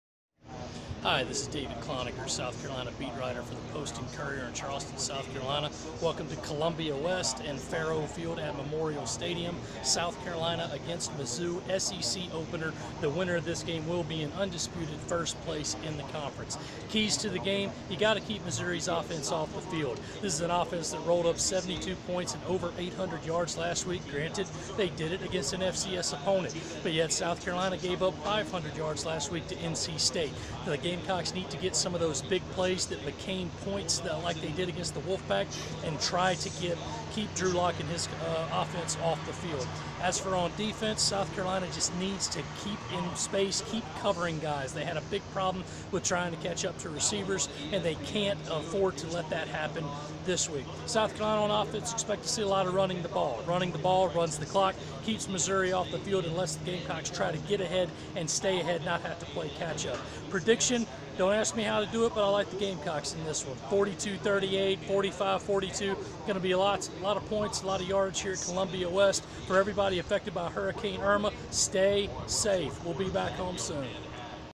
Scene-setter: USC at Missouri
Post and Courier beat writer David Cloninger talks USC-Missouri.